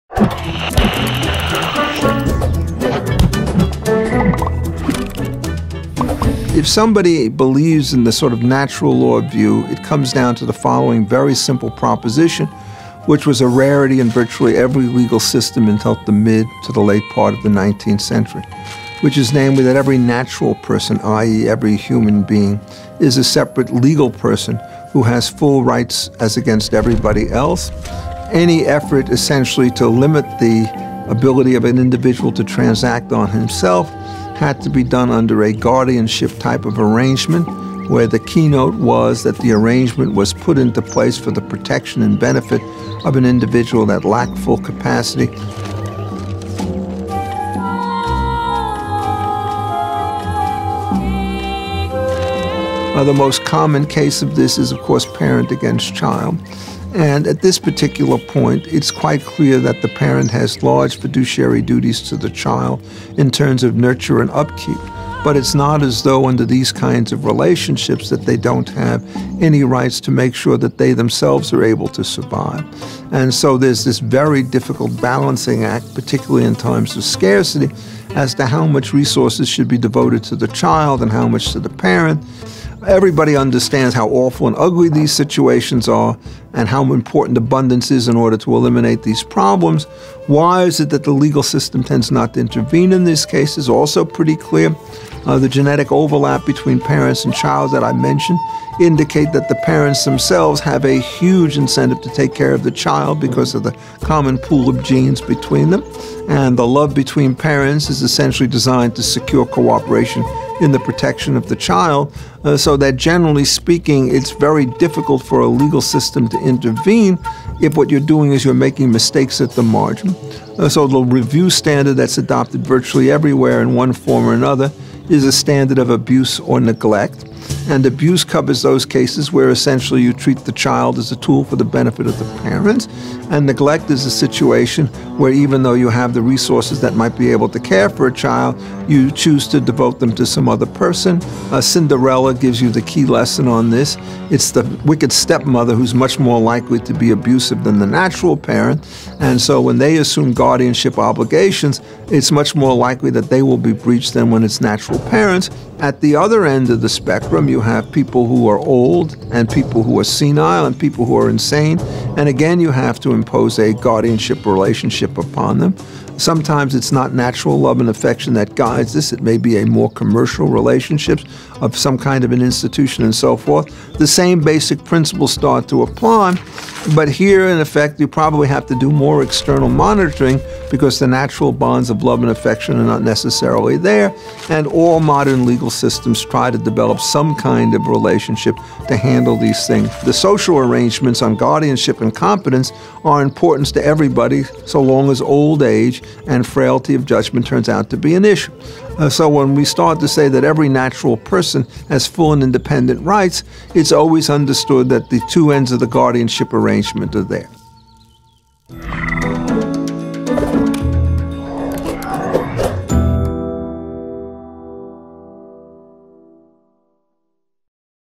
What Does the Natural Law Say About Individual Persons? Guardianship and Incompetence [No. 86]
If every person has natural individual rights, what considerations should guide decisions made by a guardian of another person?

Professor Richard Epstein discusses guardianship arrangements and duties between parents and children.  Parents are naturally inclined to nurture their children, and the legal system only intervenes if there is abuse or neglect.  Similarly, in the case of an elderly parent or incapacitated relative, the family should exercise a considerate guardianship and closely monitor any third party caregiver.

Professor Richard Epstein is the inaugural Laurence A. Tisch Professor of Law at NYU School of Law, a Senior Fellow at the Hoover Institution, and Professor of Law Emeritus and a senior lecturer at the University of Chicago.